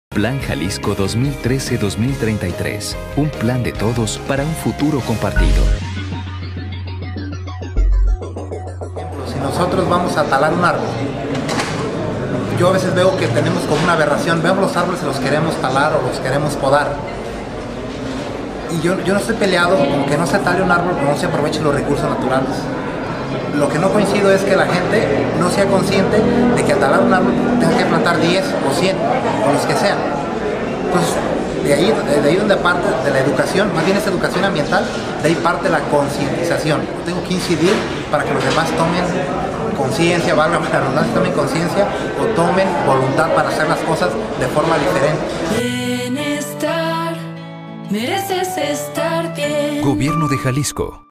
Voces de la consulta 2013, Roberto García
Entrevista a Roberto García, habitante de Tototlán, Jalisco. Participante del foro regional de la región Ciénega 2013.